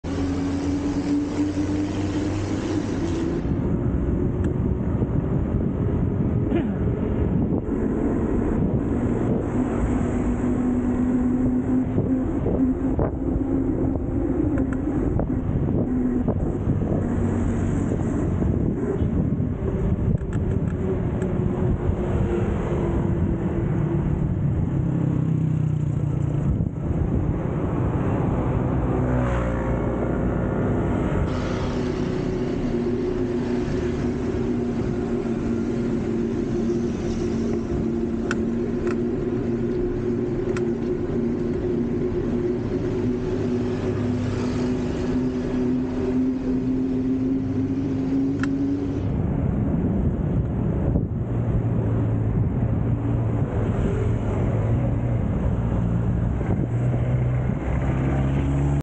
Pedaling Through the Bustling Metropolis: A Thrilling Manila Bike Ride
Join us on an exciting biking adventure through the bustling metropolis of Manila! Our guide will take you on a journey through the busy streets and iconic landmarks of the city, as you pedal past the towering skyscrapers, bustling markets, and historic sites. With its diverse terrain and bustling atmosphere, biking through Manila is an unforgettable experience that will surely leave you feeling energized and exhilarated. Whether you're a seasoned rider or a beginner, this ride offers a unique perspective of the city that you won't find anywhere else. So grab your bike and join us on this epic journey of exploration and discovery!